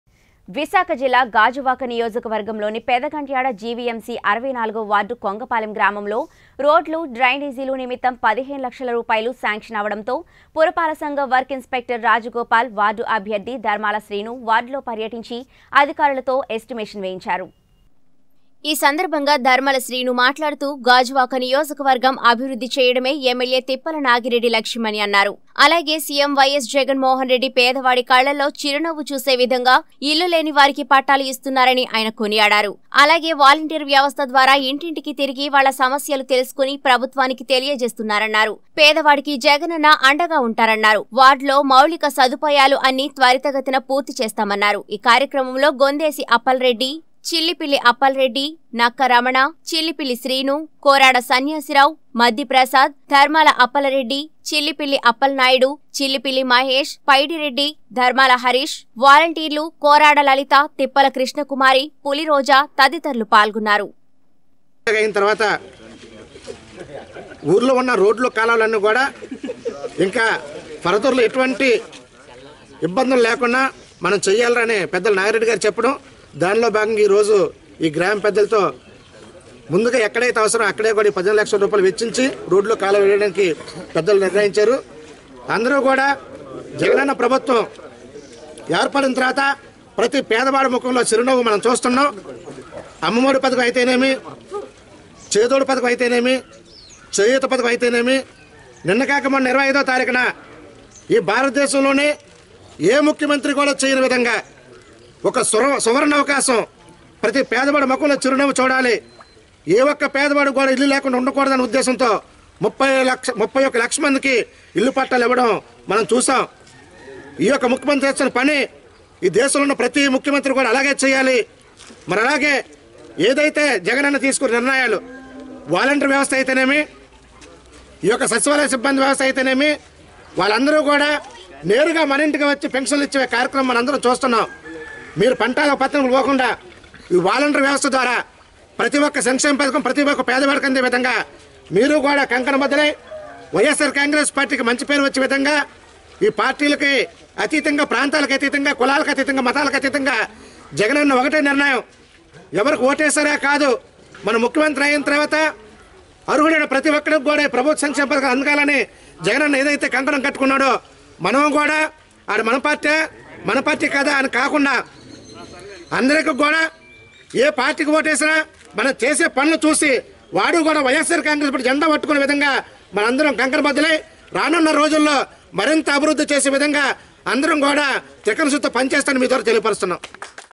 గాజువాక నియోజకవర్గం పెదగంట్యాడ జివిఎంసి 64 వార్డులో పురపాలక సంఘం ఇన్స్పెక్టర్ రాజగోపాల్ పర్యటన
విశాఖ జిల్లా గాజువాక నియోజకవర్గం పెదగంట్యాడ జివిఎంసి 64 వార్డులో పురపాలక సంఘం వర్కర్ ఇన్స్పెక్టర్ రాజగోపాల్ పర్యటన | 19 Newstelugu